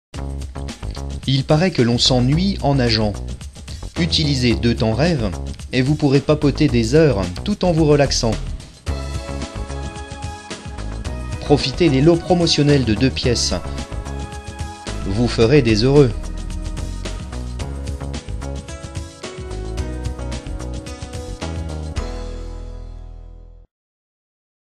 TENREV appareil de natation pour petite piscine privée
avec TENREV, on peut nager dans une toute petite piscine, même à 2 personnes pour ne pas s'ennuyer !